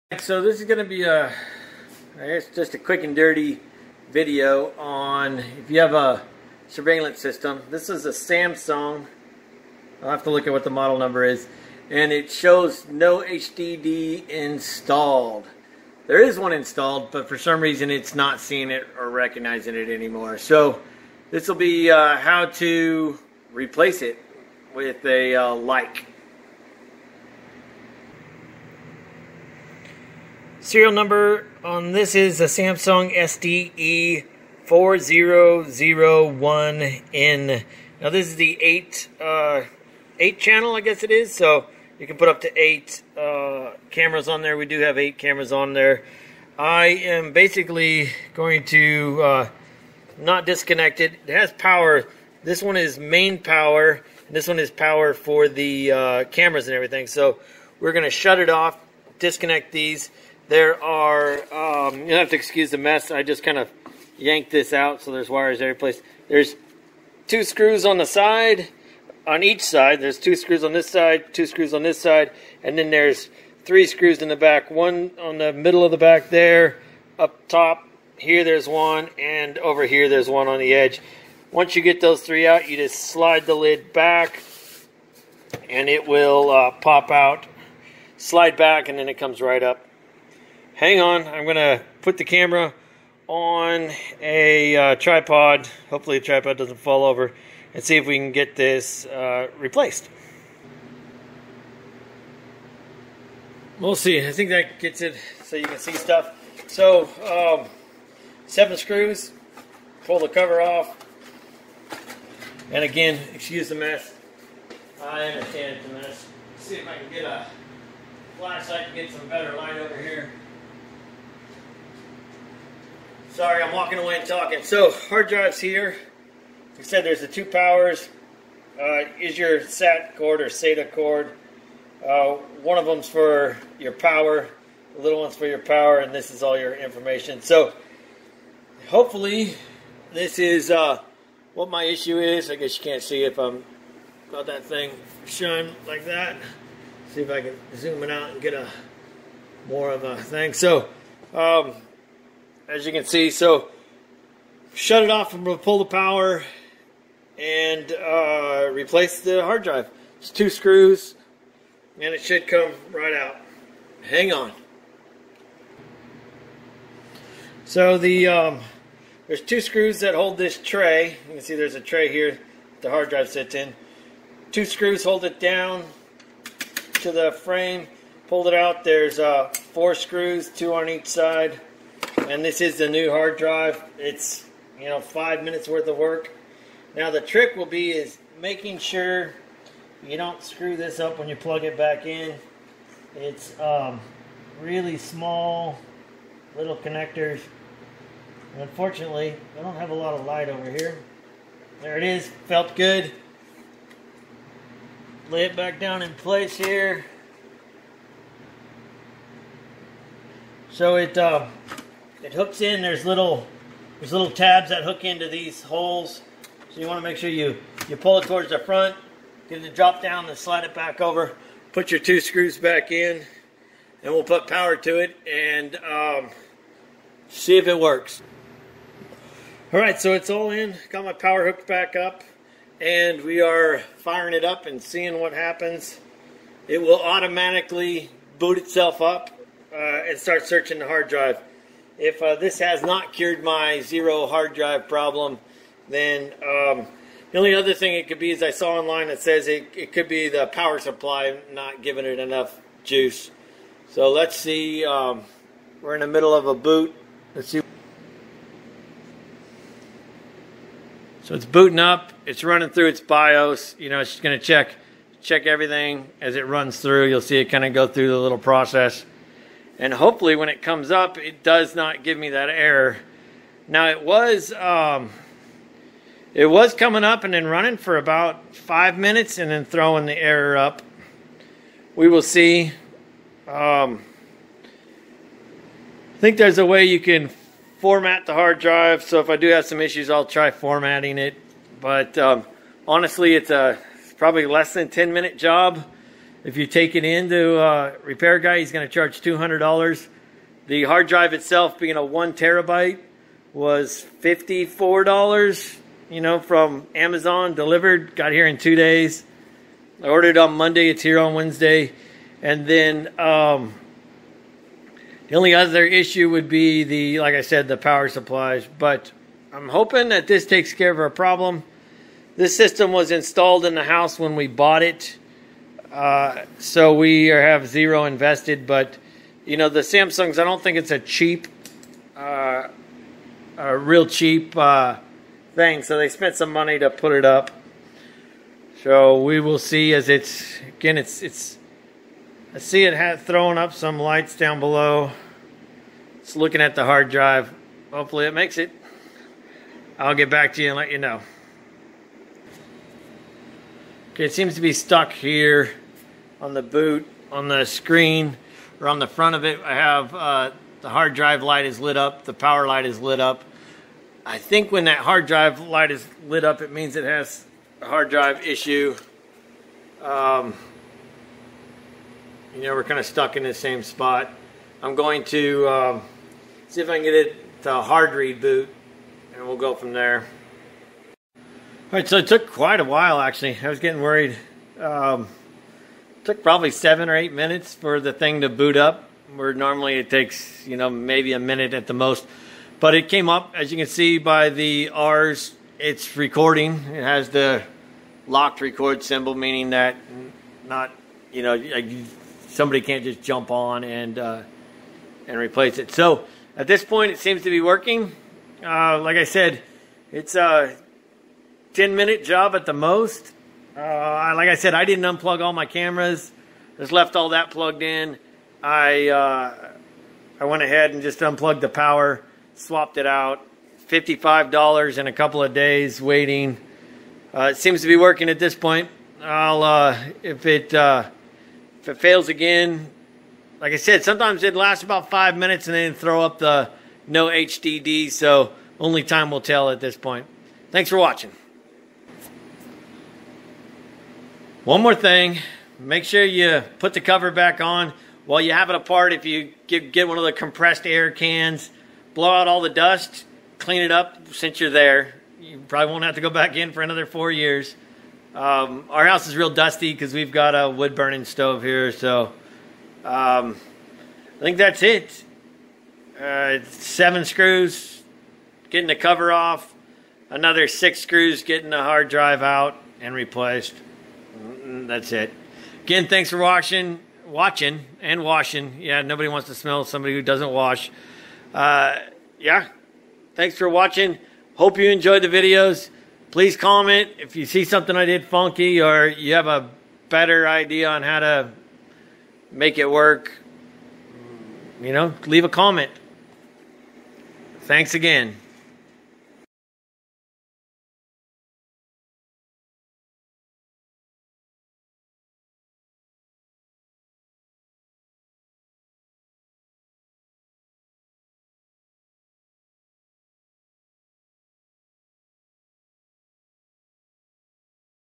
SAMSUNG SDE-4001N HDD REPLACEMENT
REPLACING THE HDD IN A SAMSUNG SDE-4001N